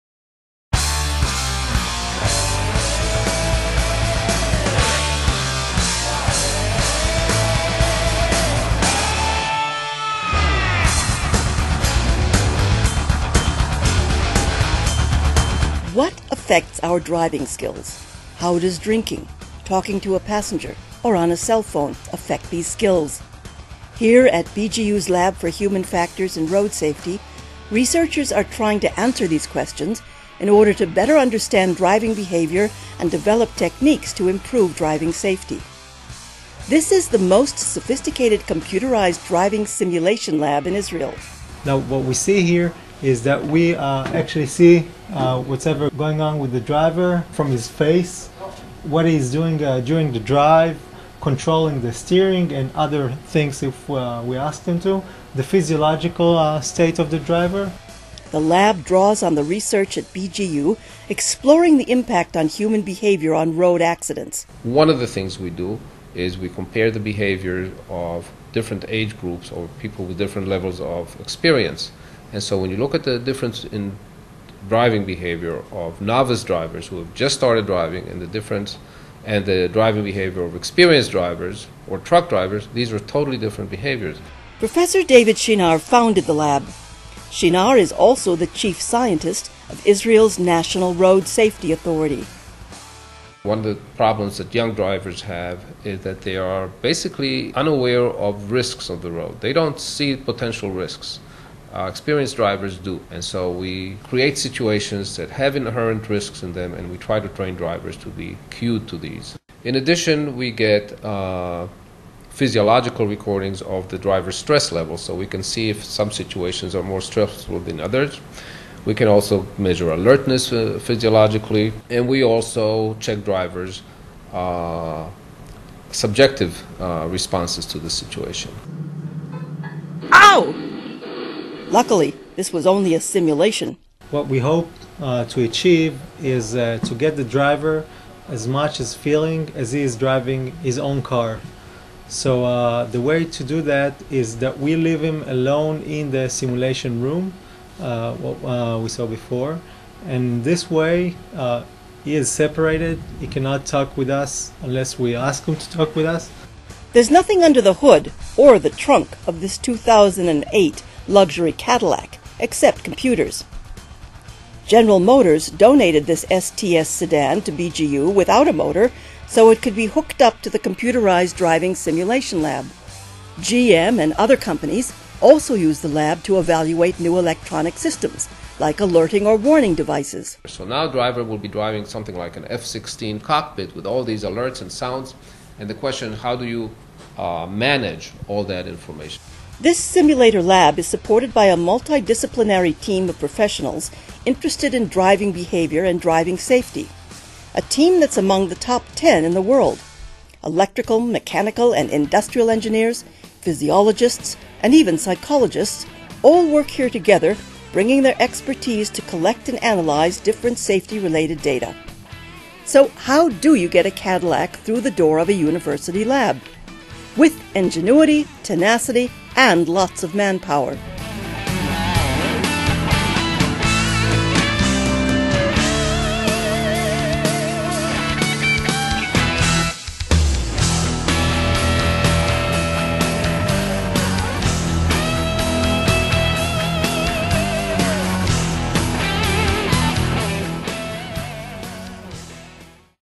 Driving Simulation Laboratory
Drivers who mix alcohol and drugs are just not safe. New research by BGU road safety experts show that even small amounts of alcohol and marijuana mixed together form a lethal cocktail for drivers.

Being under the influence of alcohol and other drugs is one of the main causes of road accidents. In the Western world today, road accidents are the leading cause of death for people aged between five and 35.

The research included 12 social users of marijuana (seven men and five women aged 24 to 29). A control group smoked cigarettes and drank juice without alcohol. Others drank alcohol and smoked tobacco, while the study group smoked marijuana and drank alcohol.

The volunteers were then asked to drive a car using a computerized simulator. They were asked to navigate straight and crooked sections, on level and hilly roads and in foggy conditions. Before and after driving, they were asked to solve two-digit addition and subtraction problems.